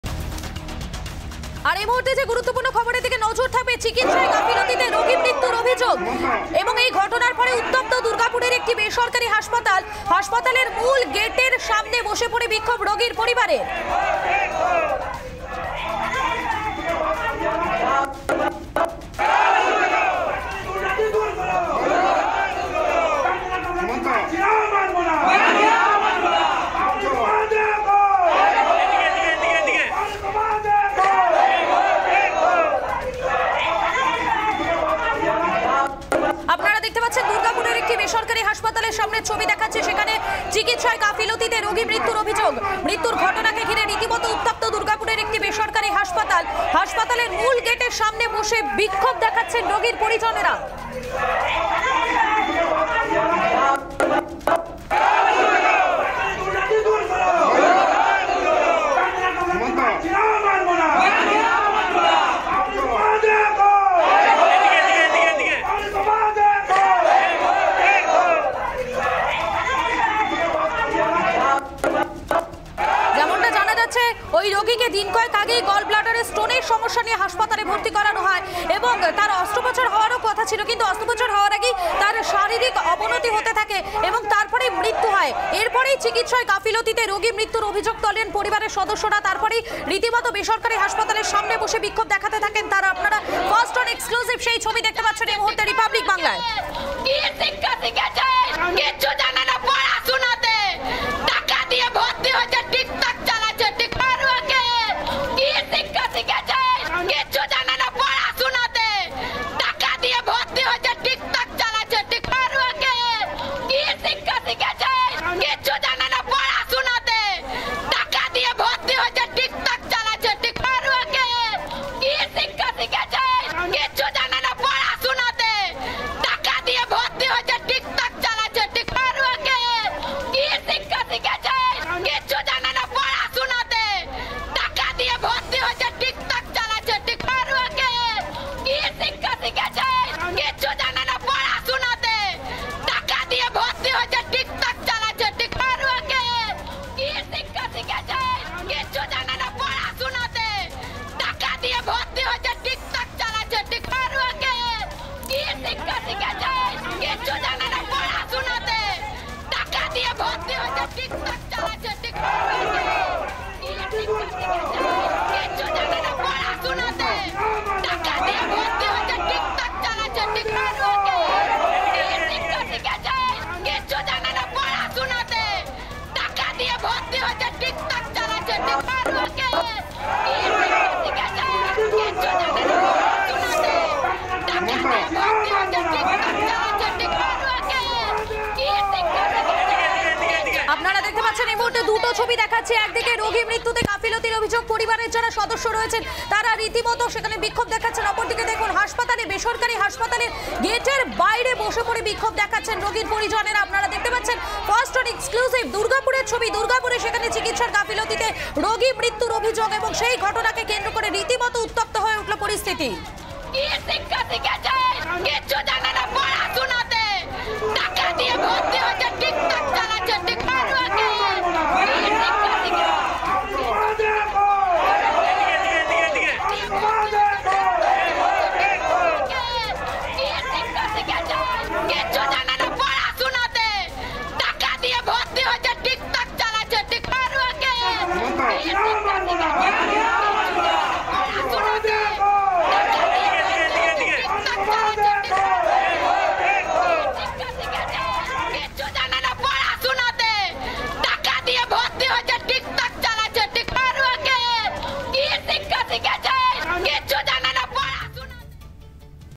Durgapur News | রোগীমৃত্যুতে উত্তেজনা দুর্গাপুরে | চিকিৎসায় গাফিলতি, রোগী মৃত্যুতে উত্তপ্ত দুর্গাপুর
Durgapur News | রোগীমৃত্যুতে উত্তেজনা দুর্গাপুরে । চিকিৎসায় গাফিলতি, রোগী মৃত্যুতে উত্তপ্ত দুর্গাপুর। বেসরকারি হাসপাতালে বিক্ষোভ

রিপাবলিক স্বাধীন।
রিপাবলিক নির্ভীক।
আপনিই রিপাবলিক।